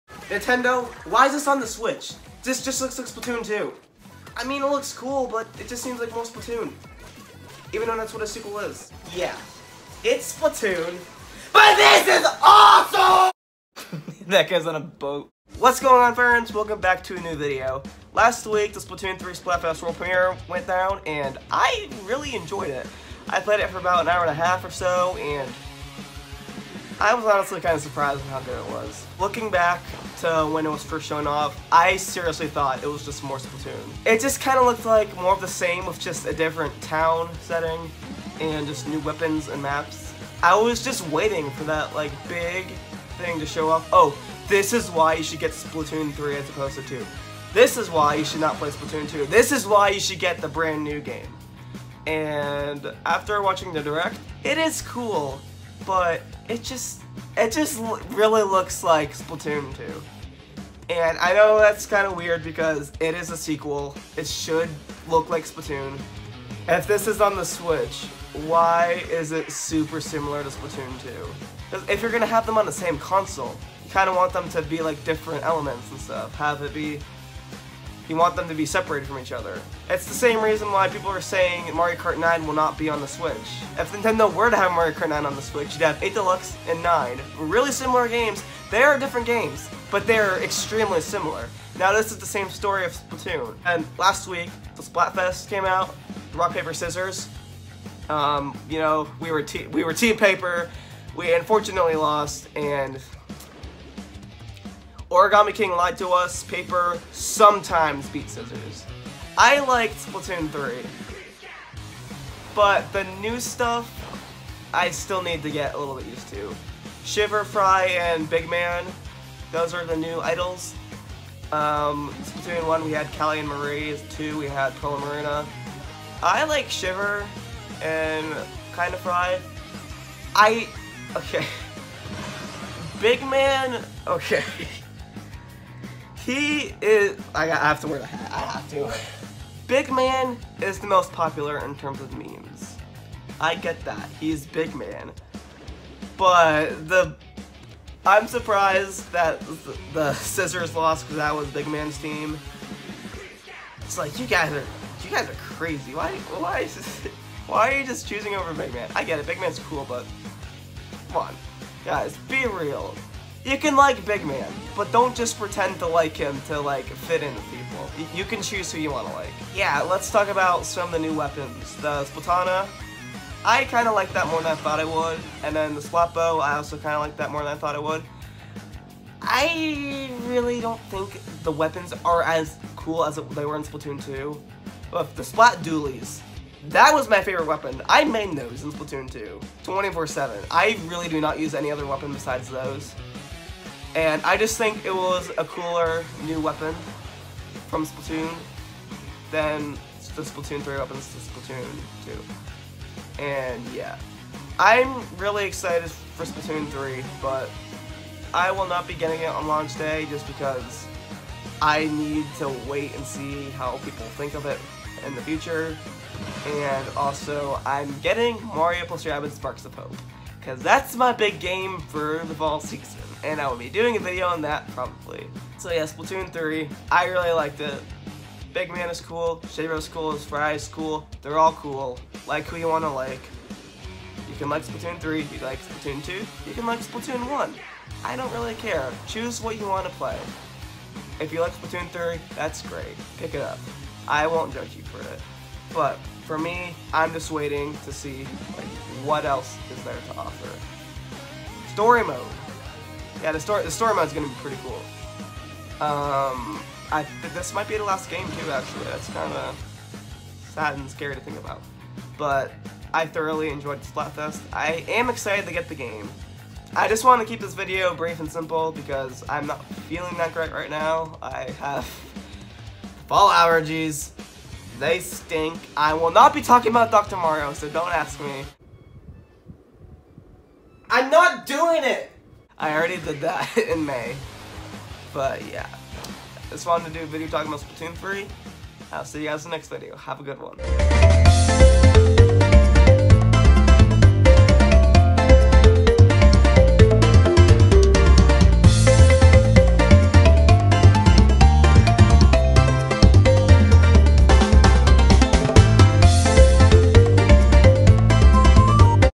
I Was WRONG about Splatoon 3! | My Honest Thoughts and Review - Antforce
I was a bit skeptical about Splatoon 3 when it was first shown off, but after playing it for awhile, it opened up to me!